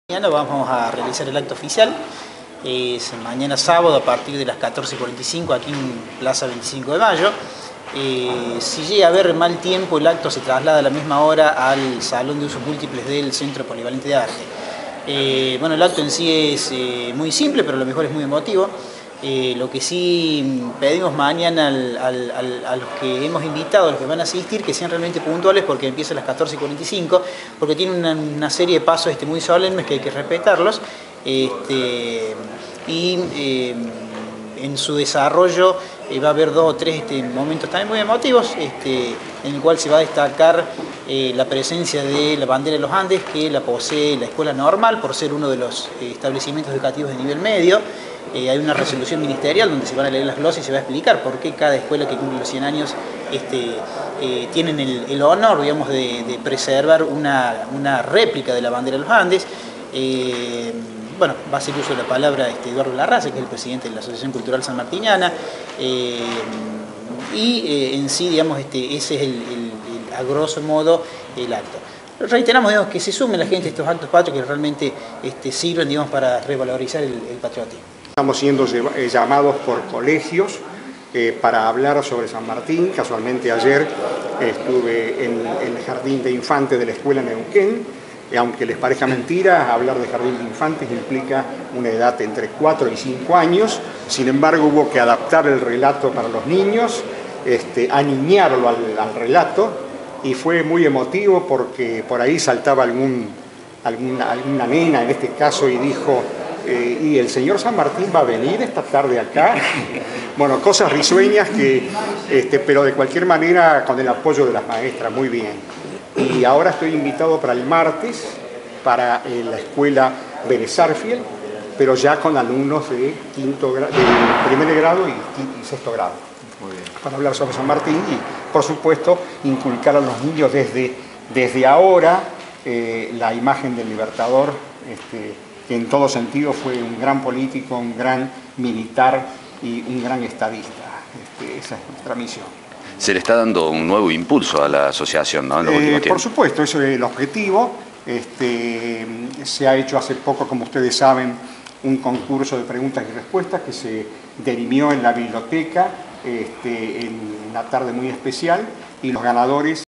BERNARDO LERDA Y EDUARDO LARRASA - ACTO OFICIAL ANIVERSARIO DEL GRAL. SAN MARTÍN EN BELL VILLE
Mañana tendrá lugar el acto oficial al padre de la patria,  del Gral. Don José de San Martín al cumplirse 236 años de su nacimiento. Así lo explican el Director de Prensa Municipal, Bernardo Lerda, y  Eduardo Larrasa, Presidente de la Asociación Cultural Sanmartiniana de Bell Ville, quién estará a cargo de las palabras alusivas.